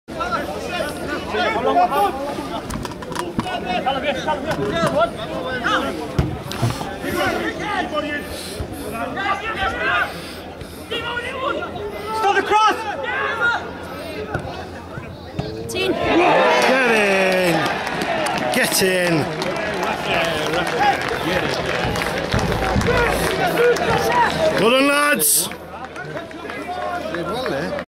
13/14 League v Newcastle Benfield 1
Spennymoor v Newcastle Benfield 01/03/2014 - Goalscorer - Keith Graydon

FT Spennymoor 1-0 Newcastle Benfield